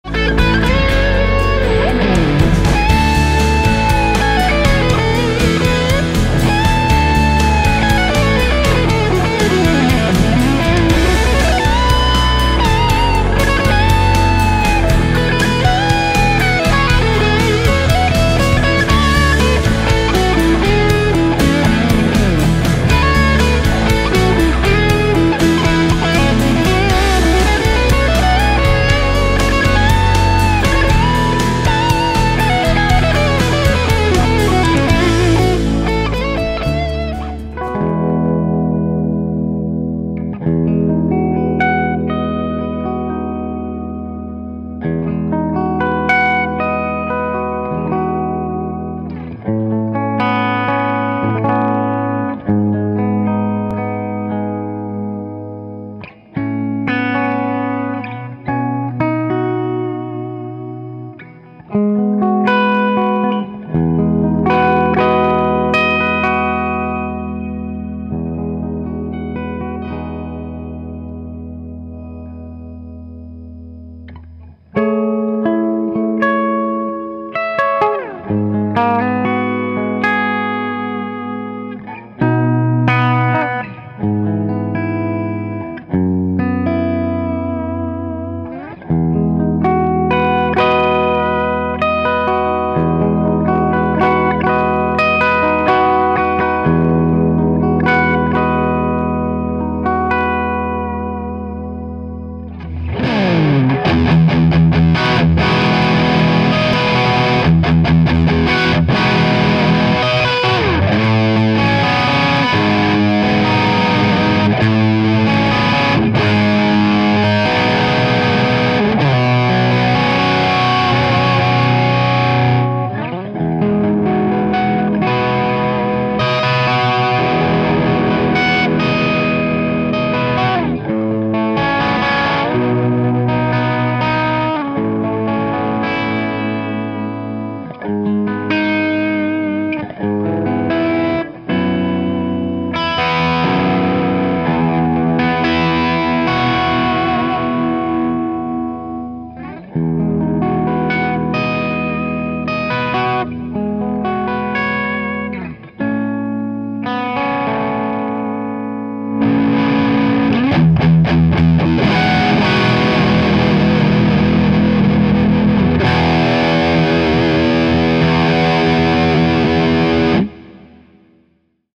Gibson Custom Shop Brian Ray '62 SG Junior | Demo by Paul Audia
Check out this guitar and its availability for purchase ONLINE at this GUITAR SAUCE link:

Chapters:
00:00 - Solo
00:37 - Clean
01:42 - Crunch

Gear:
- Custom Audio Amplifiers PT-100 Pete Thorn PT100 (solo)
- Custom Audio Amplifiers PT-100 Pete Thorn PT100 (clean)
- Custom Audio Amplifiers PT-100 Pete Thorn PT100 (crunch)